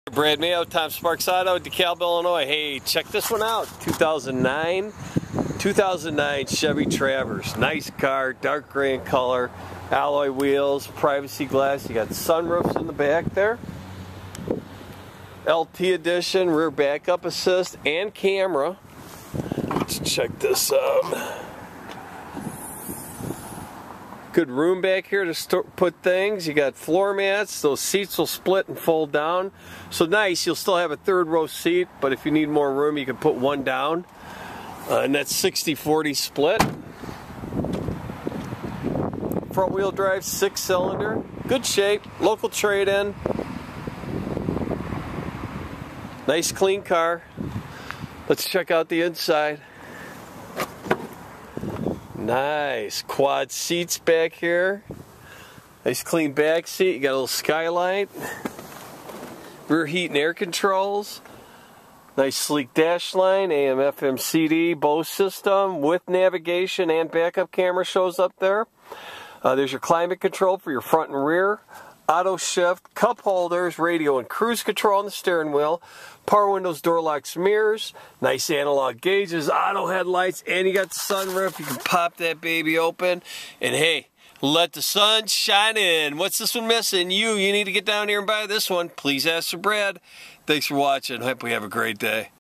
2009 Chevrolet Traverse Lt Dekalb IL near Sycamore IL
LOCAL TRADE IN, CLEAN AUTO CHECK EQUIPPED WITH; NAVIGATION SYSTEM, POWER SUNROOF, 3RD ROW SEAT, KEYLESS ENTRY W/REMOTE START, PREMIUM SOUND SYSTEM, BACK UP CAMERA, 18" ALUMINUM WHEELS AND MORE! CALL BRAD FOR AVAILABILITY.